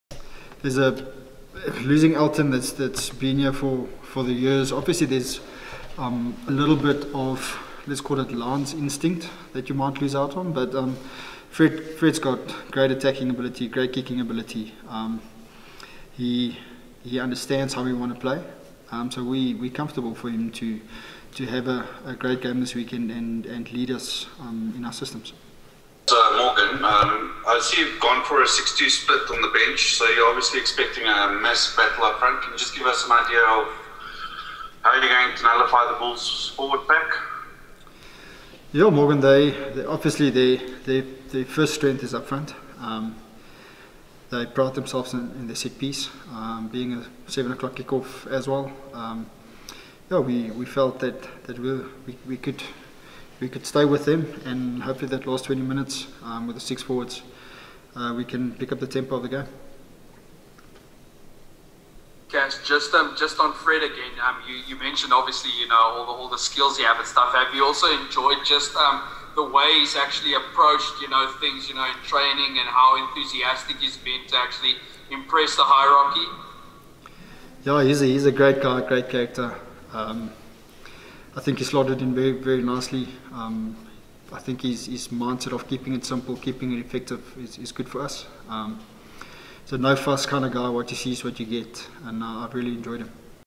Rainbow Cup: Lions coach Ivan van Rooyen on Fred Zeilinga and life without Elton Jantjies
Emirates Lions coach Ivan van Rooyen talks about Fred Zeilinga's selection at 10 and life without Elton Jantjies.